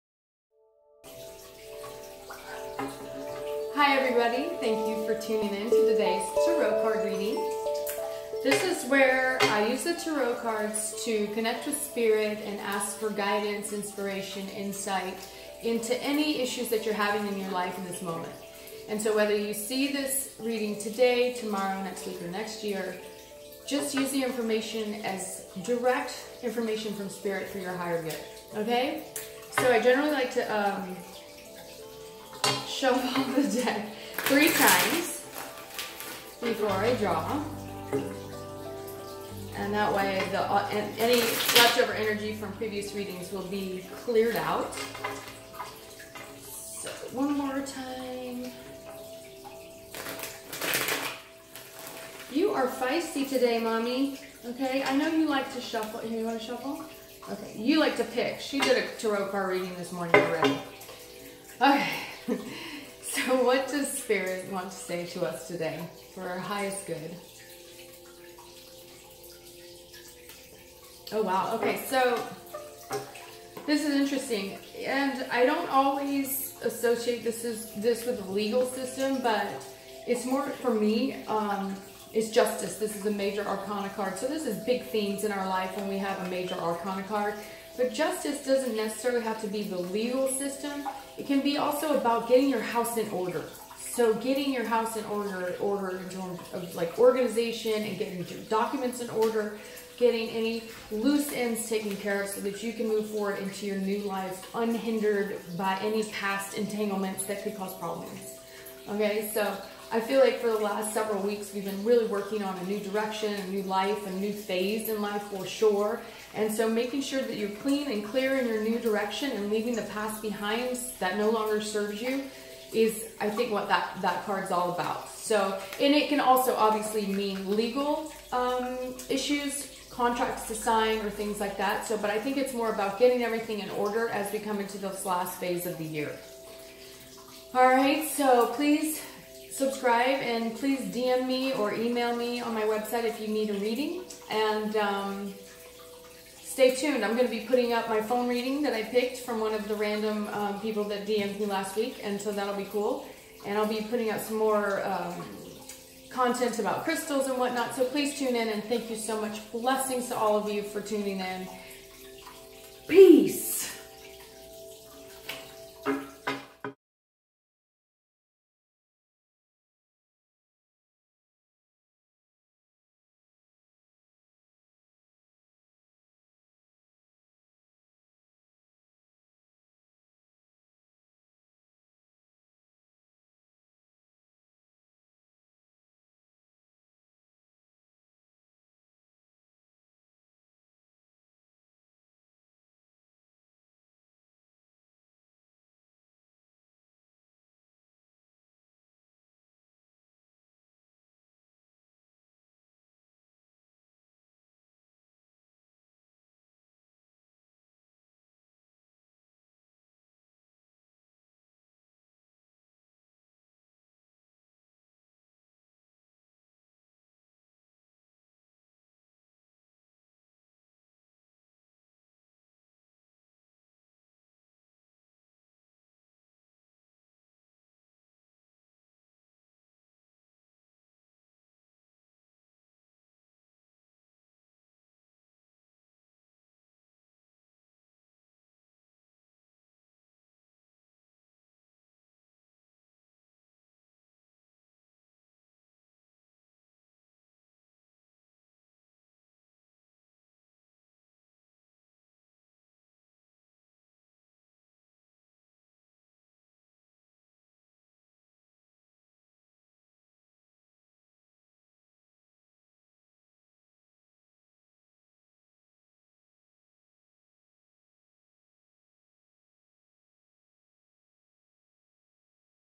Tarot Card of the Day 10 23 18
Today's Tarot Card Reading ~ Time to Clean up and get things in Order for our NEW LIFE!